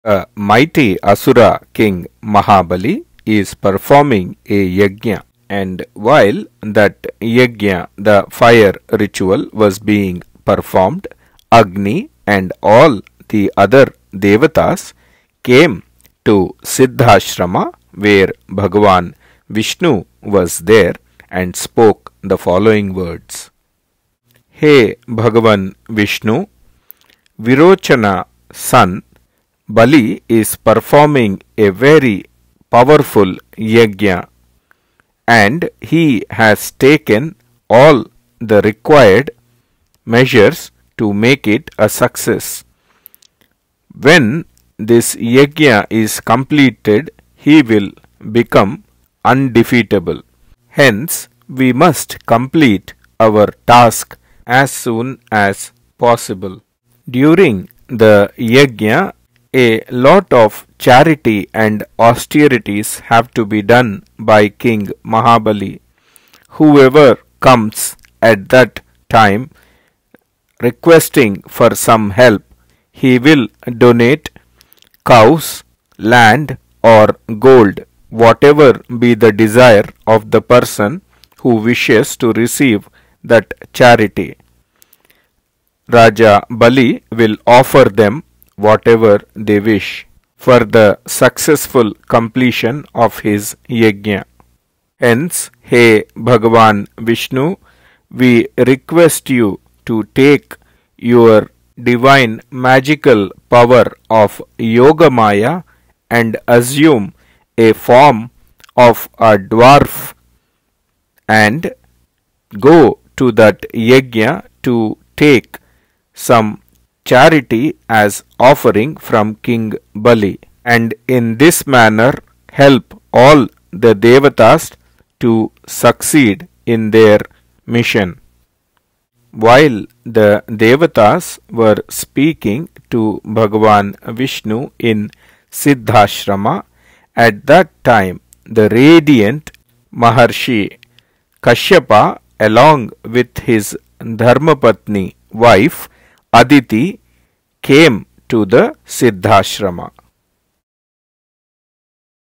Meaning Sri Valmiki Ramayana Bala Kanda 29 Sarga Shlokas 06 - 10
Sri Valmiki Ramayana Bala Kanda  29 Sarga Shlokas 06 - 10

Please listen with your eyes open,  for the  maximum benefit.

I will try and provide a simple gist in English in an easy to understand manner.

Any mistakes are mine and my limited knowledge of this epic , but I want to continue, to provide an easy to understand small size files.

Enjoy the learning process with me.

Jai Sree Sita Rama